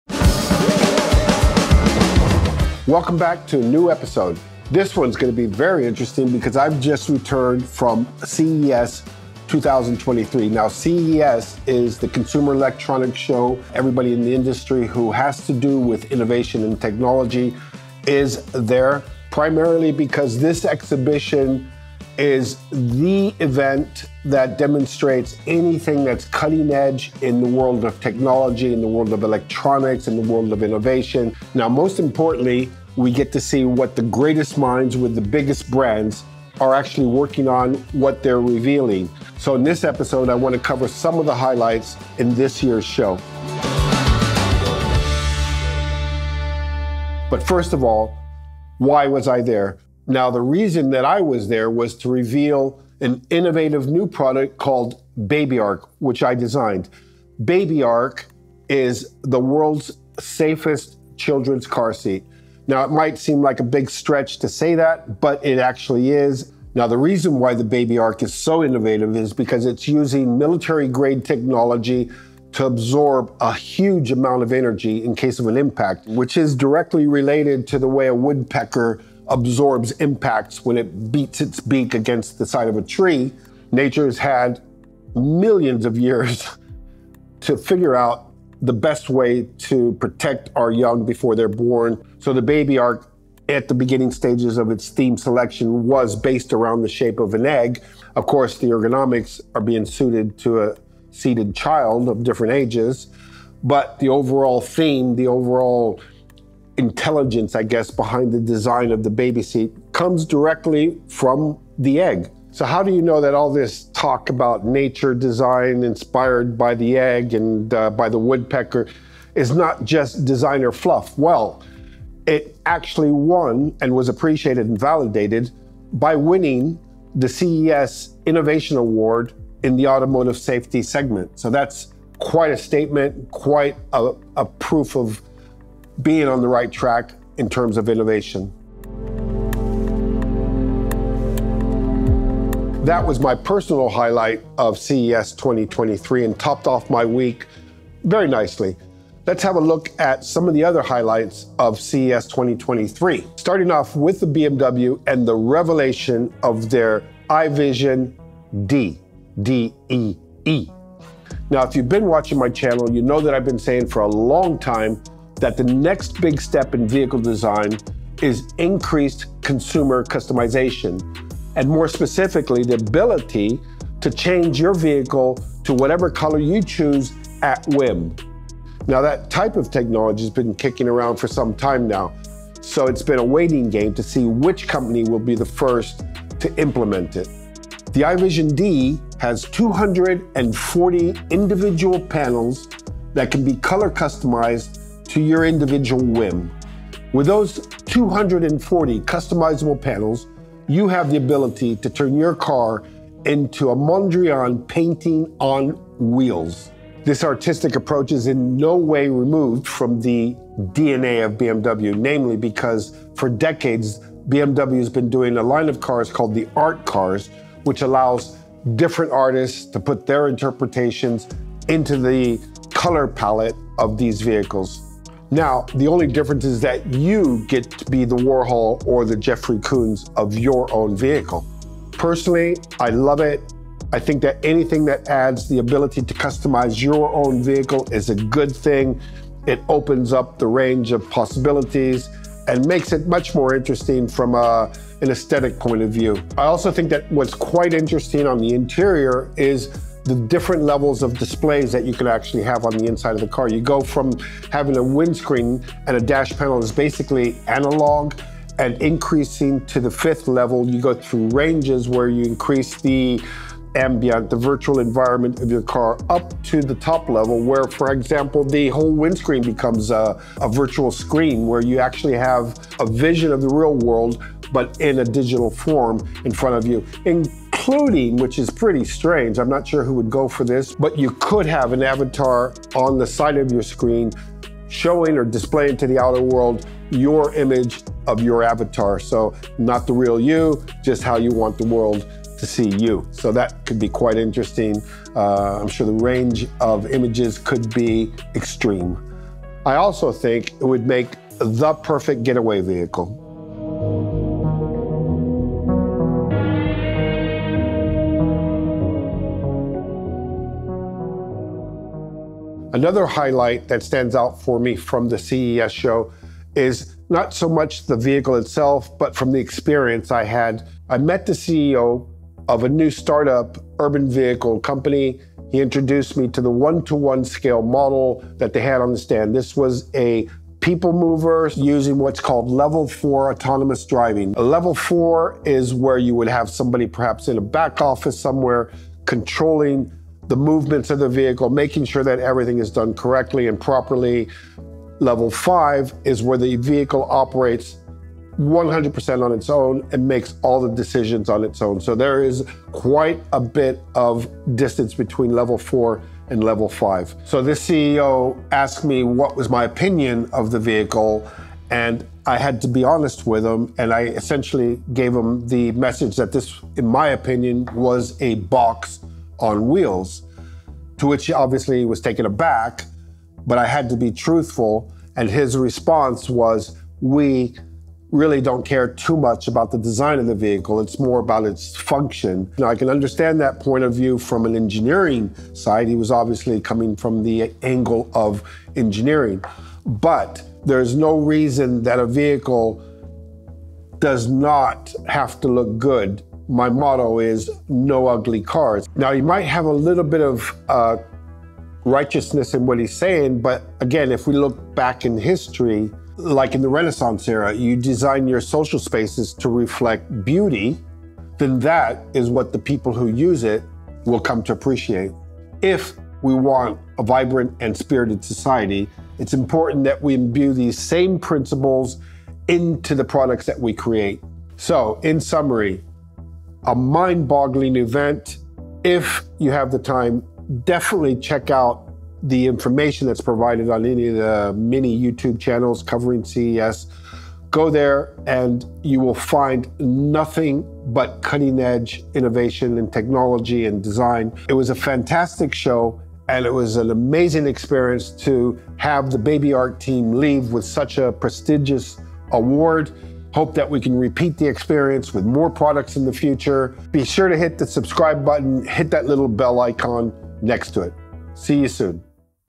What's Next For The FUTURE of Car Design? | CES 2023
World renowned designer, Frank Stephenson, reflects on his time at the worlds most groundbreaking technology convention, CES 2023.
Hi everyone,

Last week I had an the priviledge of attending CES 2023 which was a mind blowing experience about all things technology. I was there with a product I had recently designed, Babyark, but caught wind of a few eye-opening revelations while I was there.

Frank